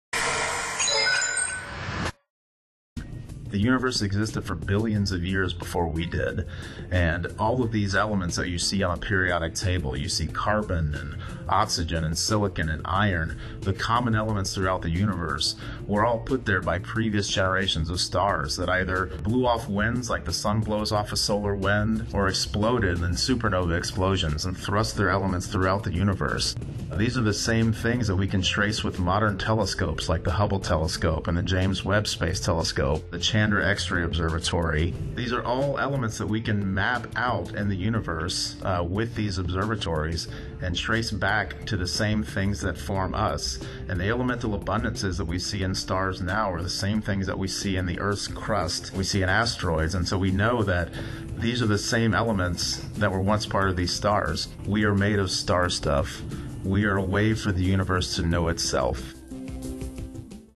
SCIENTIST: HOW ARE WE MADE OF STARSTUFF?
How are we made of star stuff? Nearly all the elements in your body were forged in ancient stars long before Earth, the Sun or our galaxy even existed. When those stars exploded, they sent their material out into space, scattering elements like carbon, oxygen and iron across the universe. Billions of years later, those same ingredients came together to form everything we know — including you.